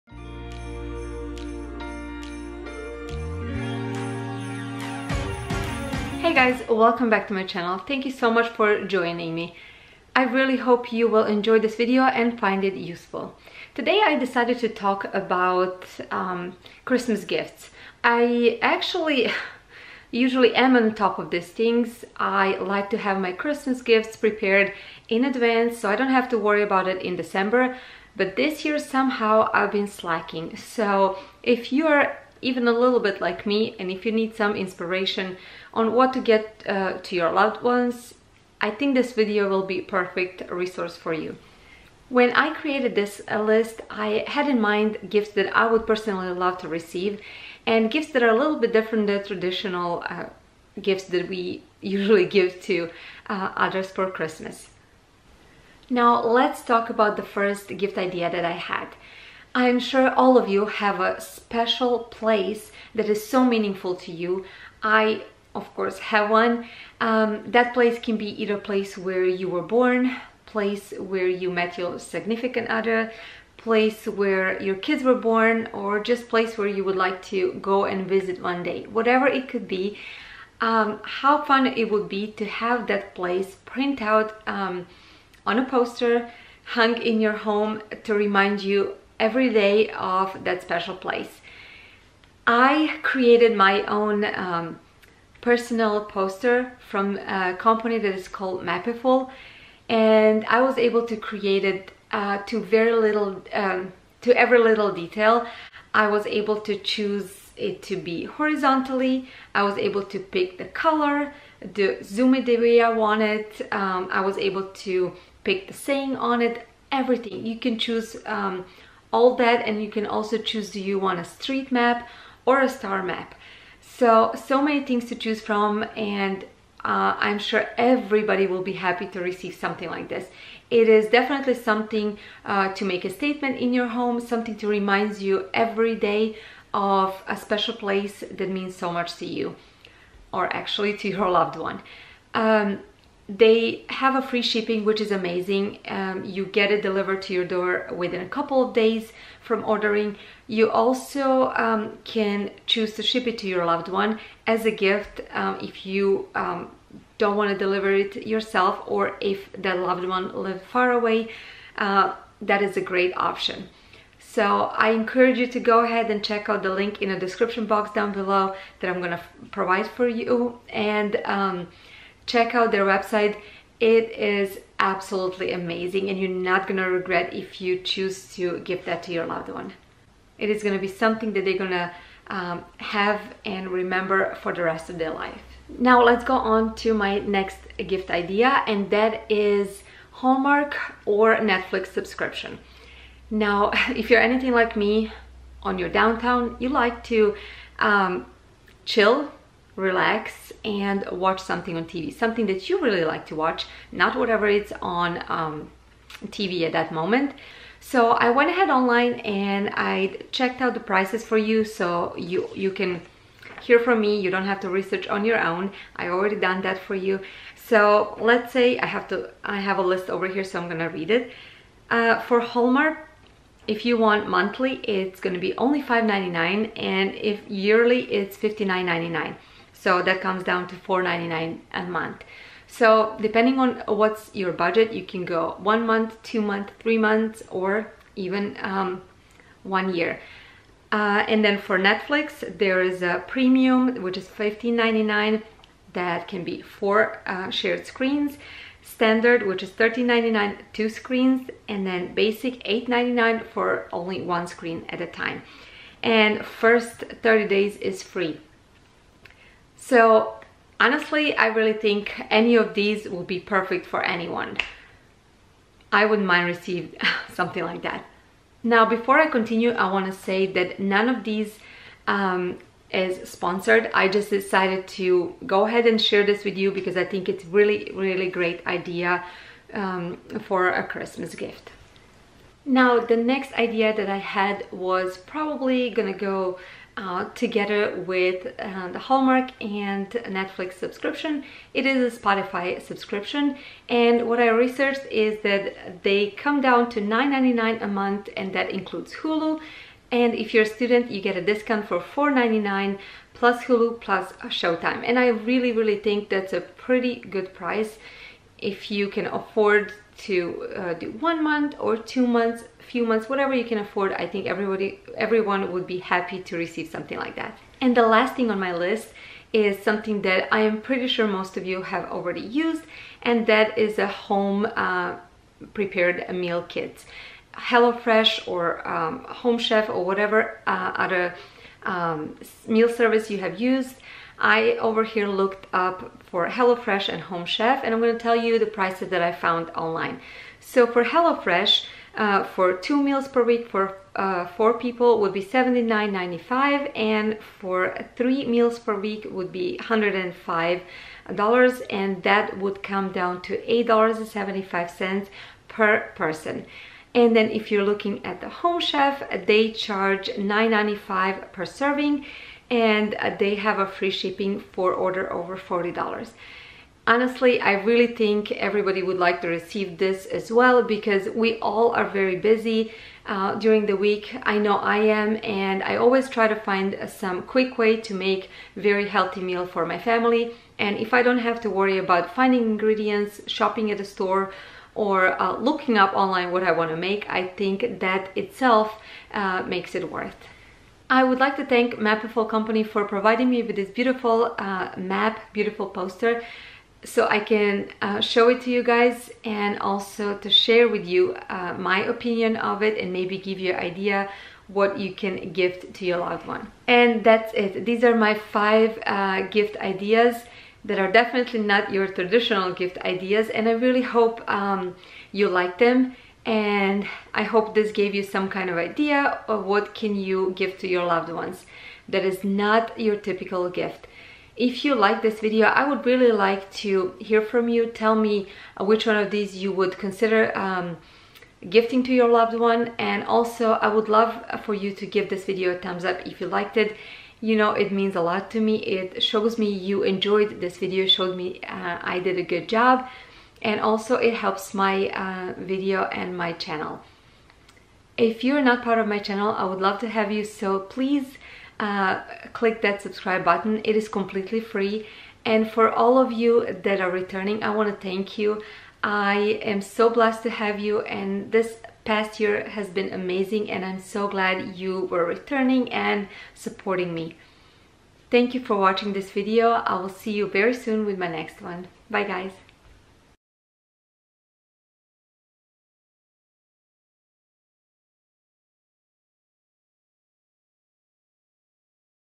Best Non traditional gift ideas // Christmas Gift Guide // Mapiful
Today I will share with you some nontraditional gift ideas that are perfect for Christmas or any other occasion!
One of them is Mapiful, who was nice enough to provide me with a sample to show it to you guys!

*Take advantage of their current discount of 25% and worldwide shipping!

*All footage in this video are my own.